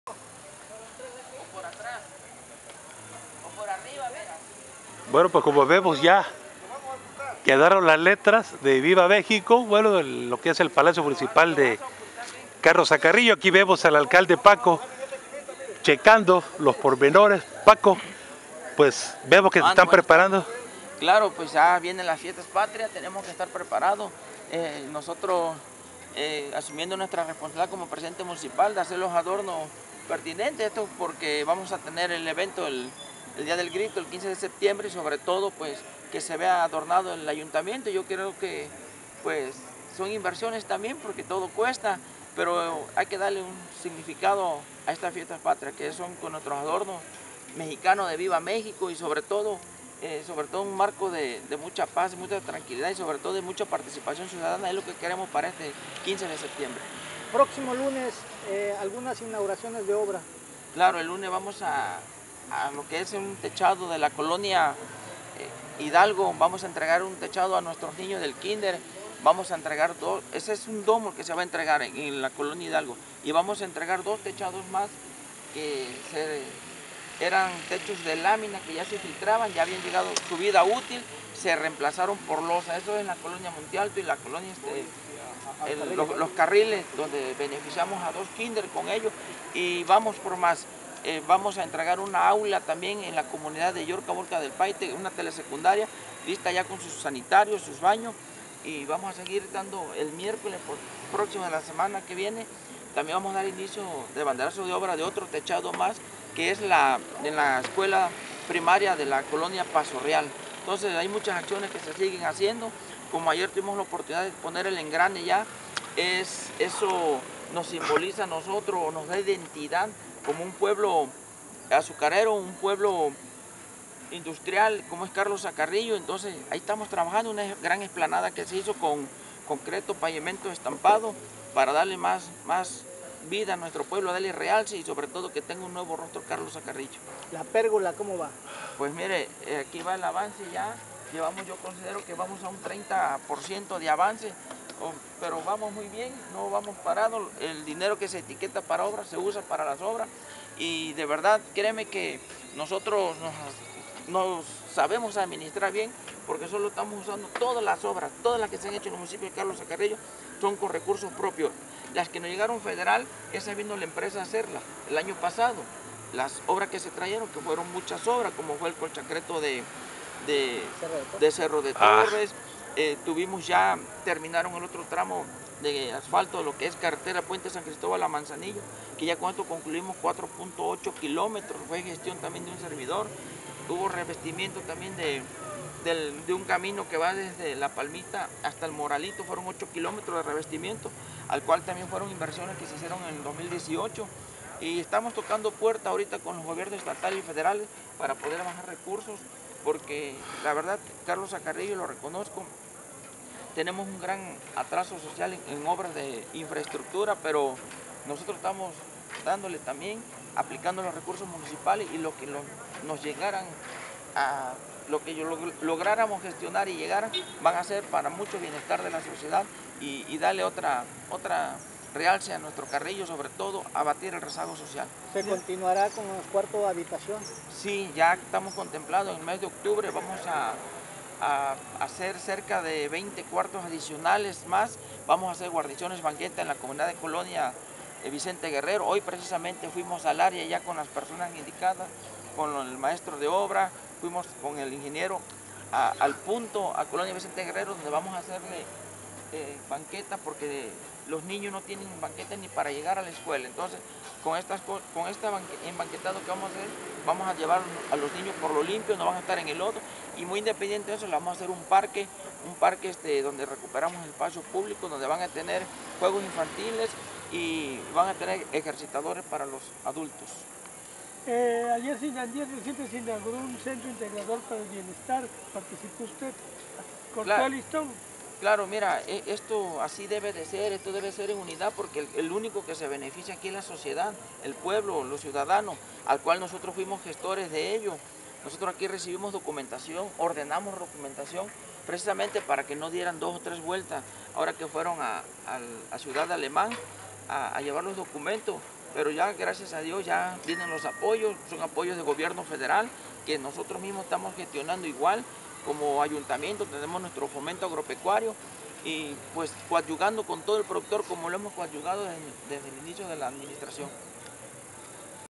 Inicia Paco Molina Preparativos De Fiestas Patrias En Carlos A. Carrillo.
Al acalde Francisco Javier Molina Arrioja  personalmente supervisó los trabajos de ornato del Palacio Municipal con motivo de las Fiestas Patrias 2019. En entrevistas habló de las obras realizadas y por realizar.